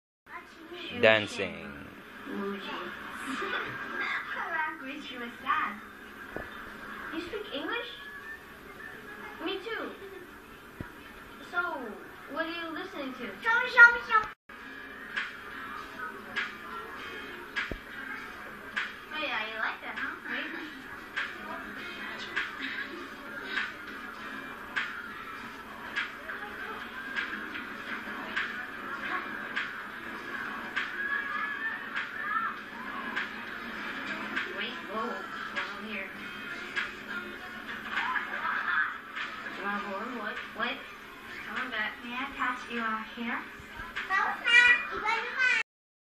Raeden Ala Drei
Raeden again mimicking Drei..